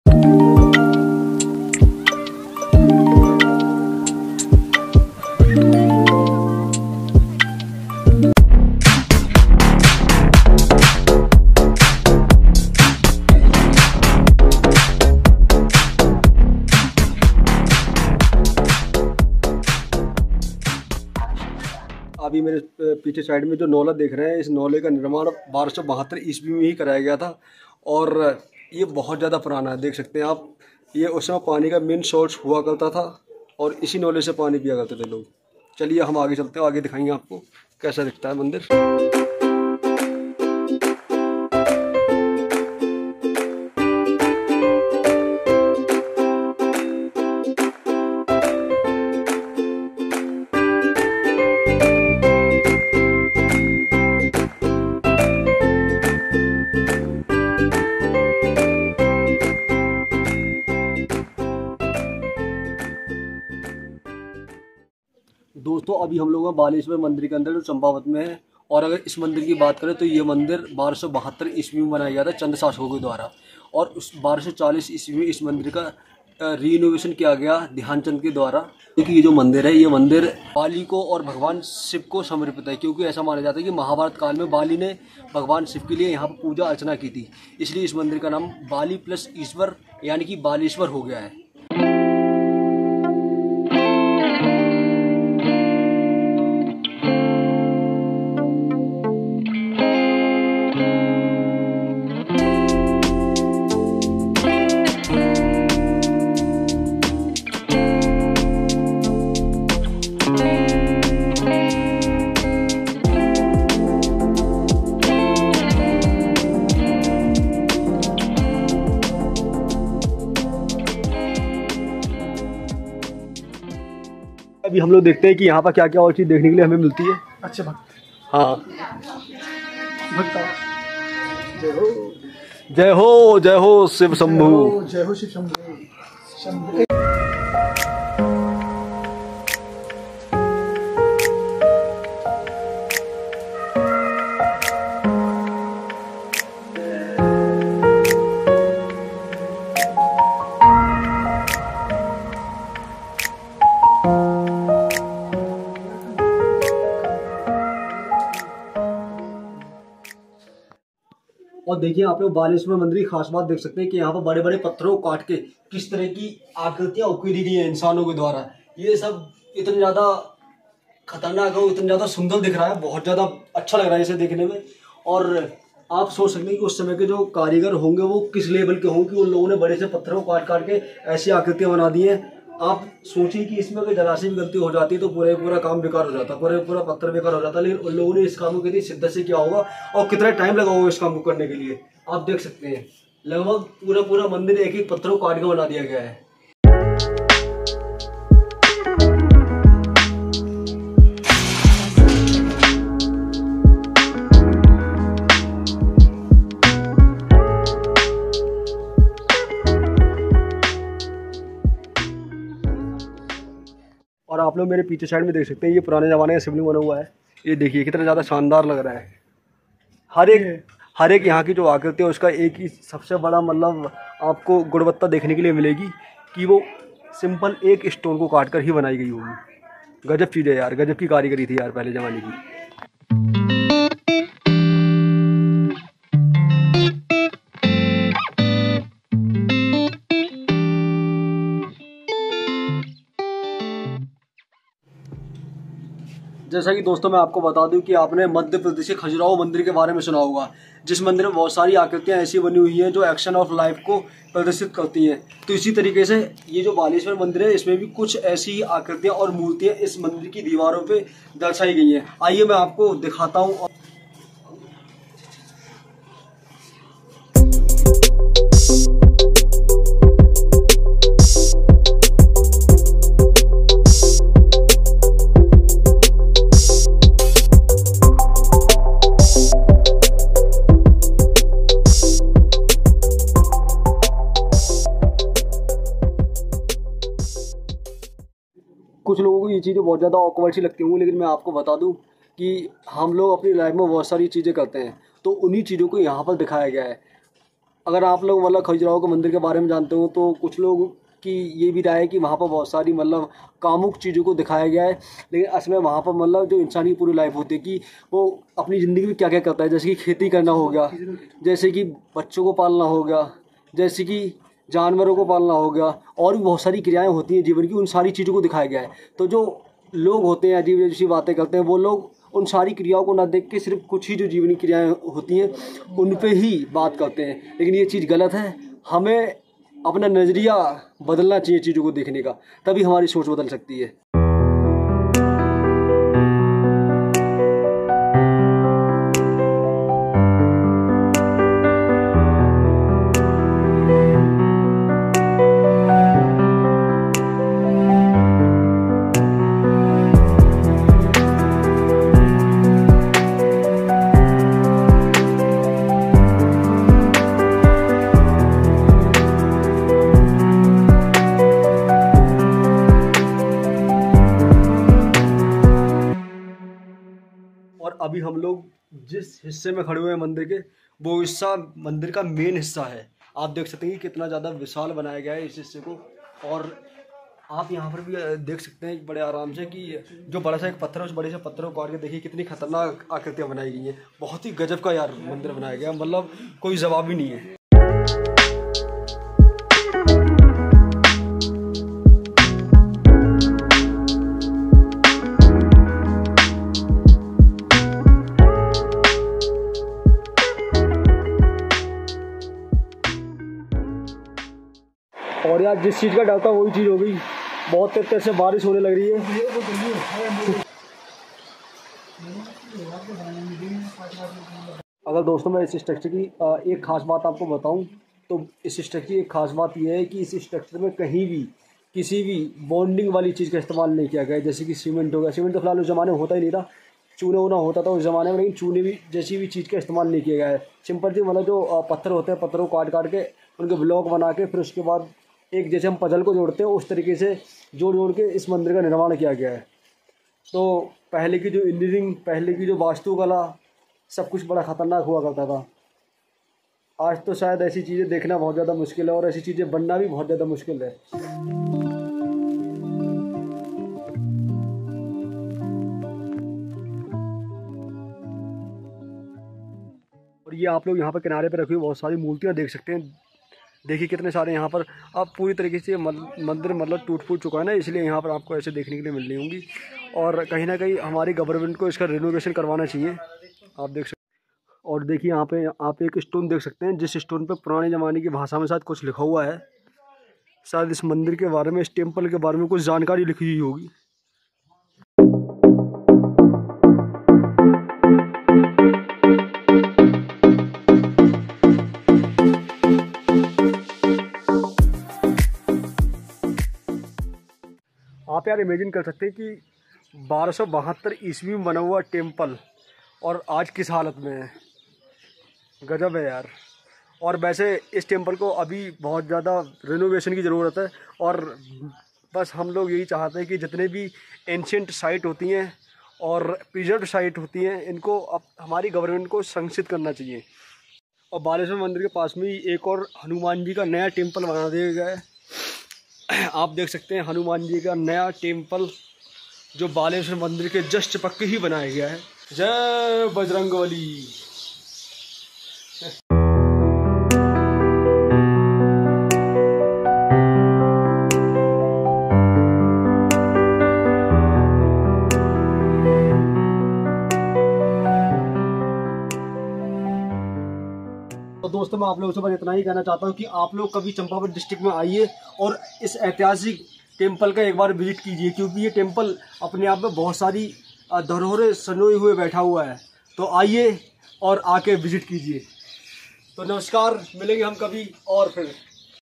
SHREE BALESHWAR MAHEDEV TEMPLE|| CHAMPAWAT(Uttrakhand)|750 year old temple||SOORYA ZONE
In this video we went to visit Baleshwar Mahadev Temple which is located in Champawat District Uttarakhand and it is 750 years old and this temple was built in 1272 AD by Chand Dynasty. in 1480 renovated by king Dhyanchand who belongs also chand Dynasty.
this temple is colletion of civil art and many of hardwork. so you can visit this temple once in a life and feel the history of uttarakhand and their glory.

pls follow on Instagram for new updates---